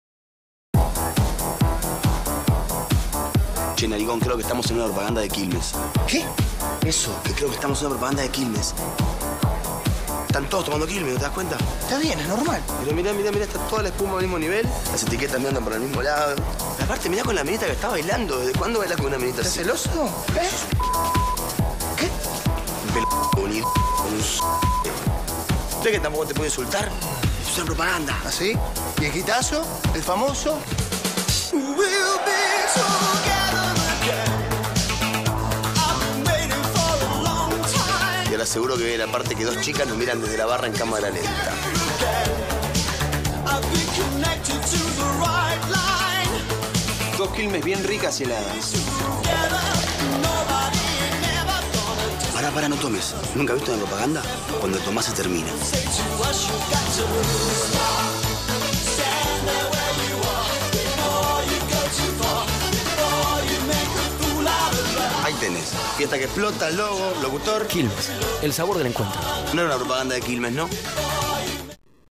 Quilmes Commercial (2009 - Argentina)
The advert titled Spot was done by YOUNG & RUBICAM advertising agency for QUILMES BEER (CERVECERIA QUILMES company) in Argentina. It was released in the August 2009. Business sector is Alcoholic drinks.